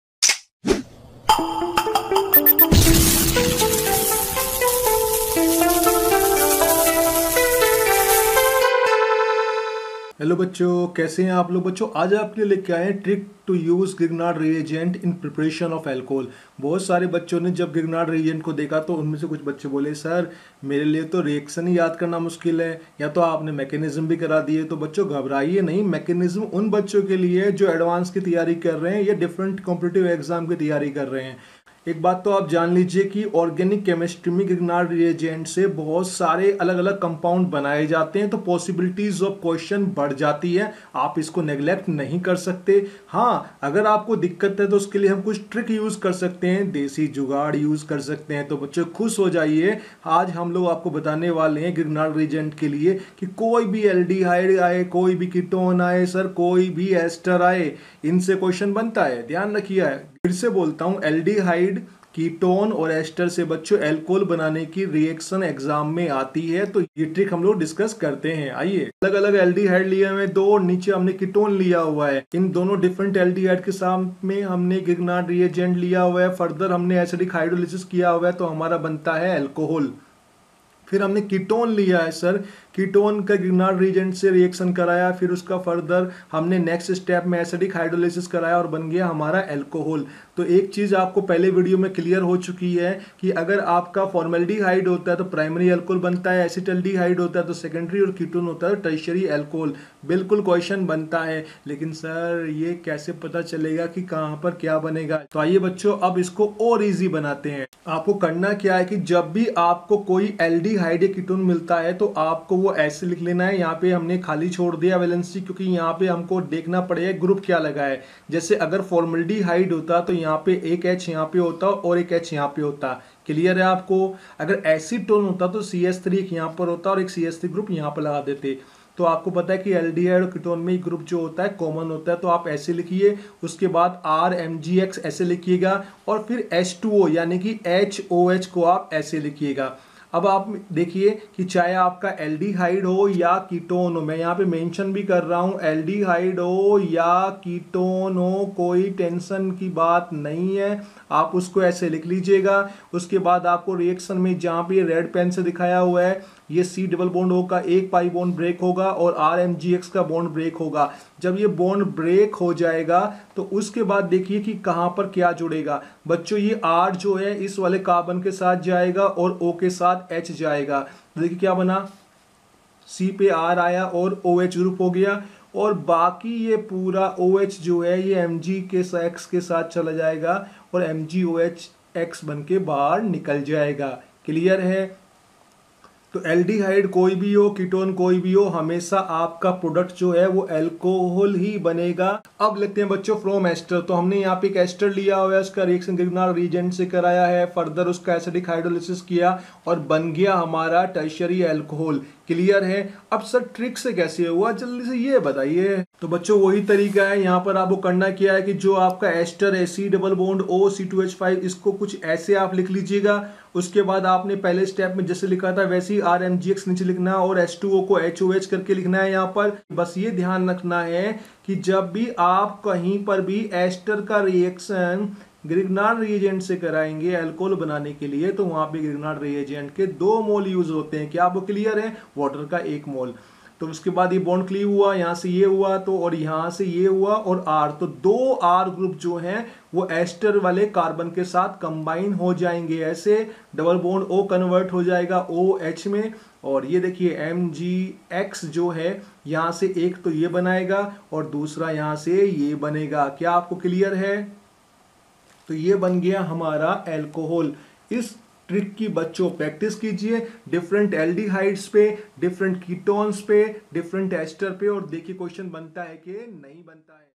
Trick to use Grignard reagent in preparation of alcohol, Chemistry Junction
In the video, trick explained to find product from the reaction of Grignard reagent with aldehyde, ketone and ester.

Chemistry Junction